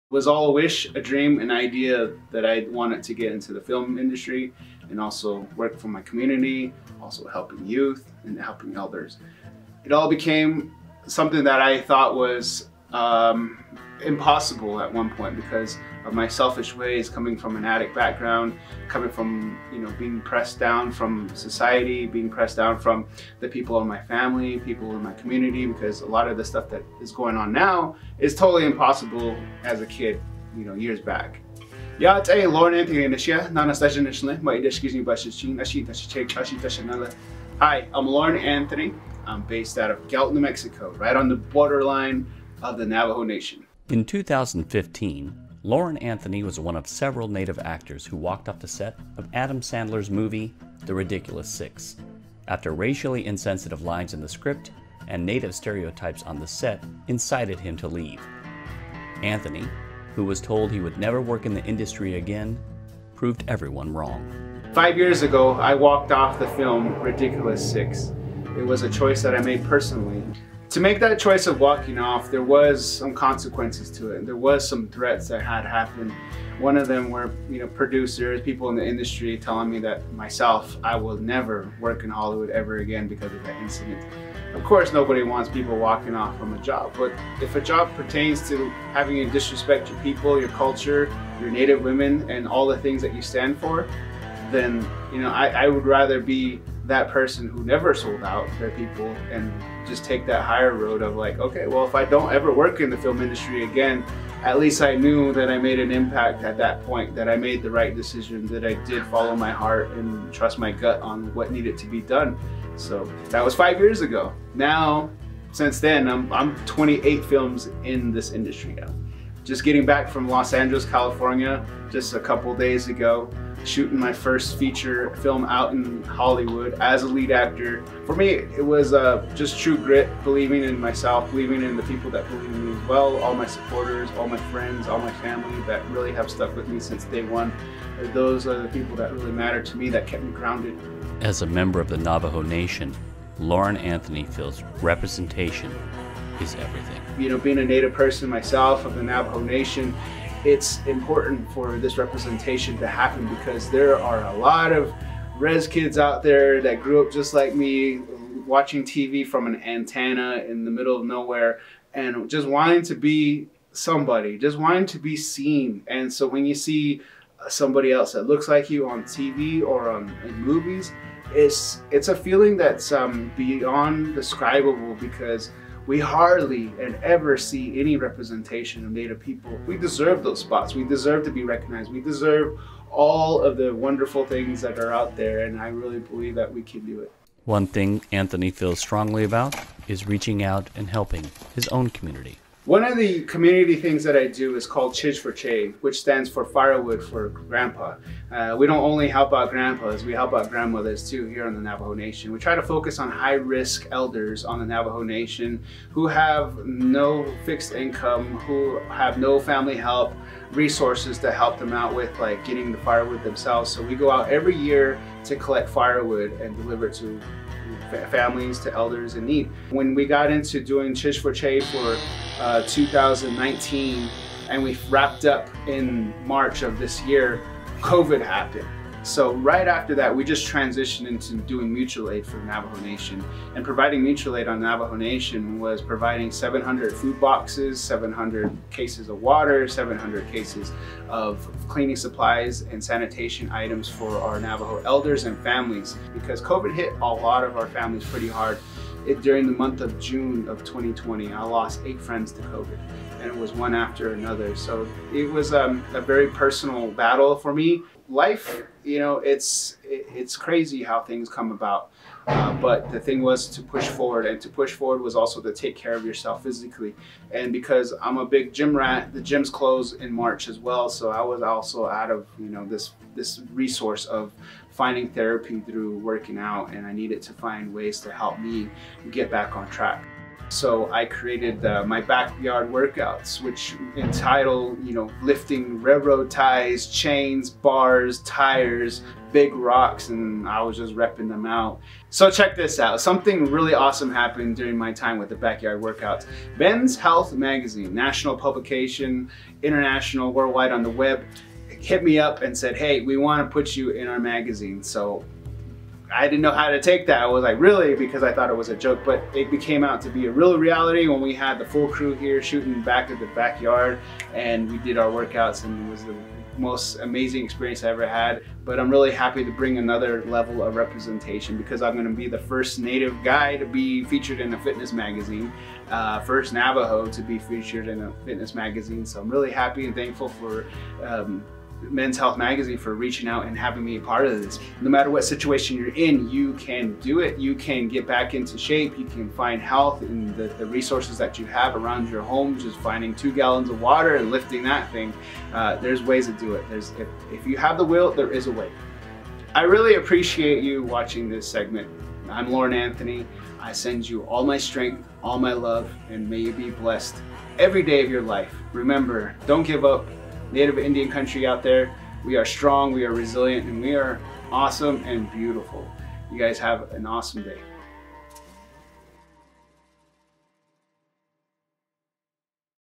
HIGH-PROFILE NATIVE ACTOR: Loren Anthony reflects on walking off set of "Ridiculous Six"
Loren Anthony has been in the spotlight during his career as an actor of the Navajo Nation. Everything from 'Meet the Millers' to 'Longmire' to 'The Lone Ranger', his resume is stacked. He was one of the actors who walked off the set of Adam Sandler's "Ridiculous Six" movie prompting worldwide outrage.
"Five years ago, I walked off the film of "Ridiculous Six". It was a choice that I made personally. To make that choice of walking off, there was some consequences to it, and there was some threats that had happened. One of them were producers, you know people in the industry, telling me that, myself, 'I would never work in Hollywood ever again because of that incident'," Anthony said.

Vincent Schilling (Akwesasne Mohawk) produced a video piece on Anthony's experience in Hollywood.